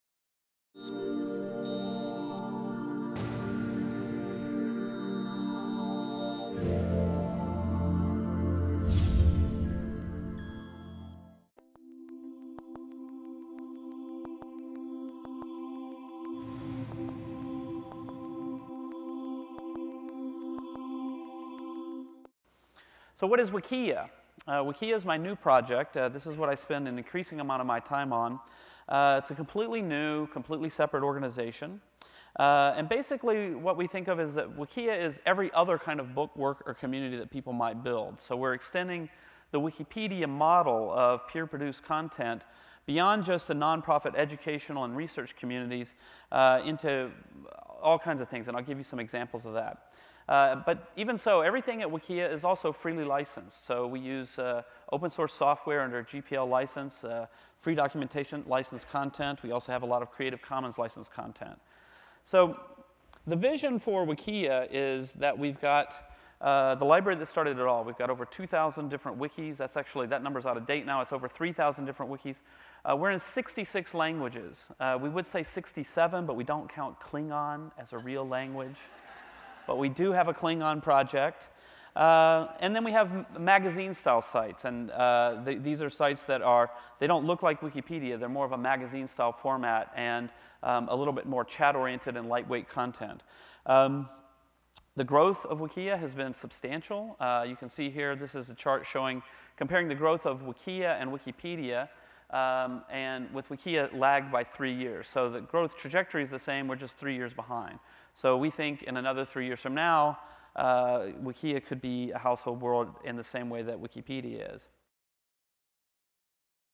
From Jimmy Wales Keynote - Discussing Wikia
In this clip, Jimmy Wales,founder of Wikipedia and keynote speaker at The Market Research Event - 2007, describes his new effort, Wikia.